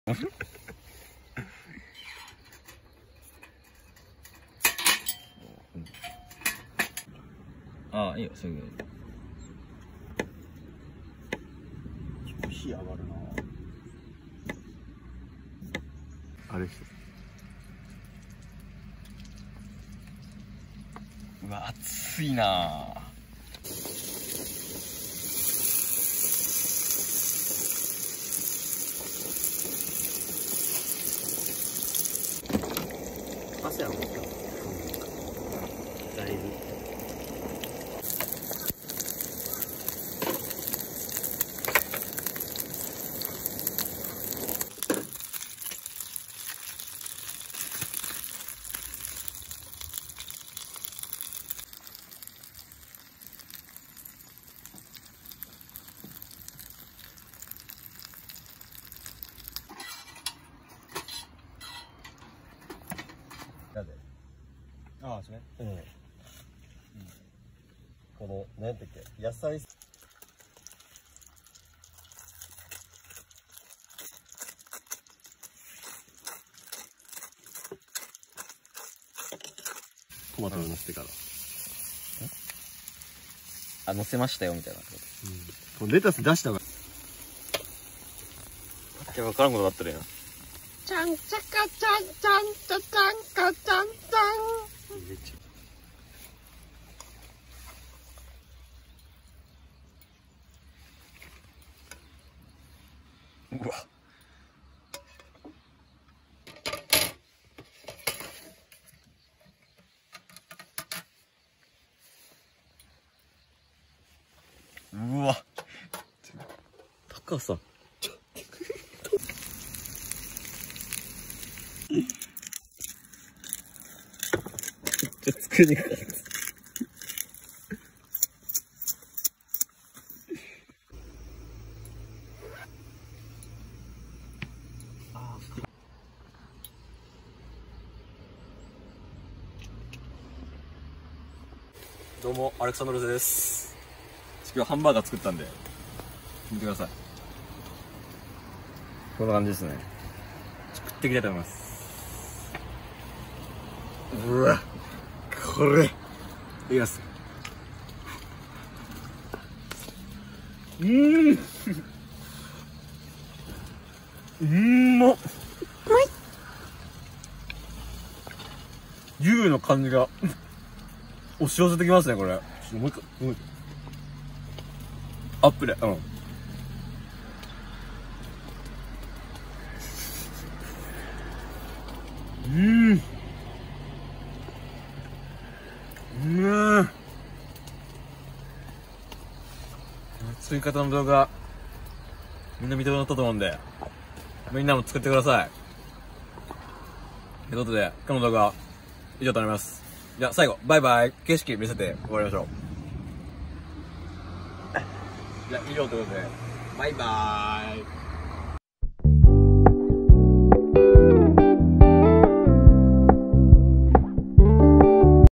キャンプ場で手作りドデカハンバーガーをかっ食らう！！！
キャンプ初心者です！